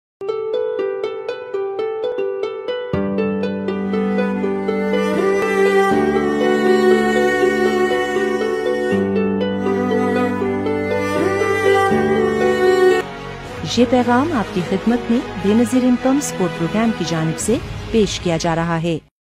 Government of Pakistan distributing Rs 25000 per family help flood victims with the support of BISP
سیلاب سے متاثرہ علاقوں میں بینظیر انکم سپورٹ پروگرام کی جانب سے کیمپ سائٹس فعال ہیں اور متاثرہ خاندانوں میں 25,000 روپے فی خاندان تقسیم کیے جارہے ہیں۔ اگر آپ کا خاندان ابھی تک رجسٹر نہیں ہوا تو 8171 پر اپنا شناختی کارڈ نمبر بھیجیں اور رجسٹر ہوجائیں۔ ‏⁦‪FloodsInPakistan